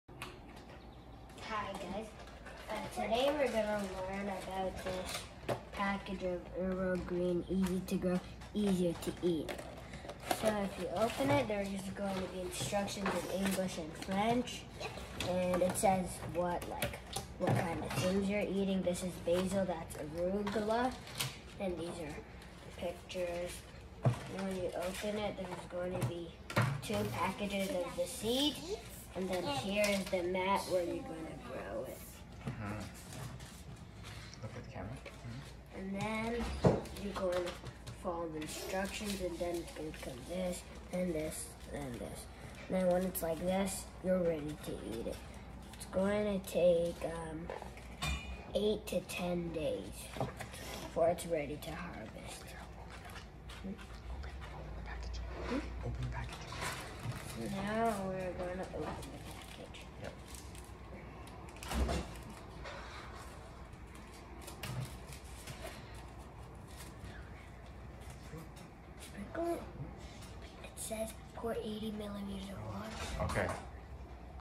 Full review and unboxing of Basil Plant Hydroponics (UgroGreens.com)
Let me know if its informative or helpful because I need to improve my YouTube presentations.